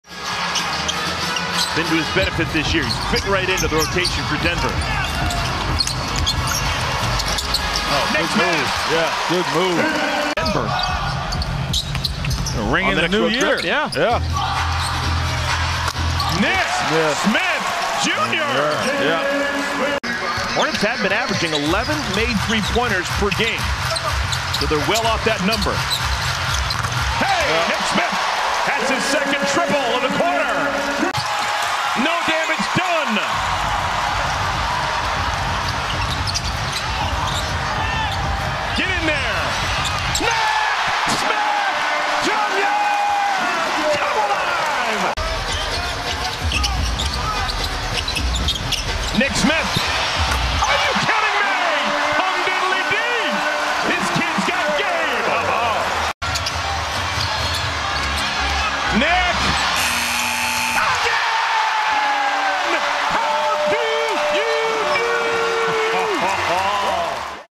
Nick Smith Jr. CAREER-HIGH 19 Points & 5 Threes vs. Nuggets! | Highlights | December 23, 2023
Nick Smith Jr. totaled a career-high 19 points and 5 three-pointers in the Charlotte Hornets' loss to the Denver Nuggets. 17 of Nick Smith's 19 points came in the 4th quarter!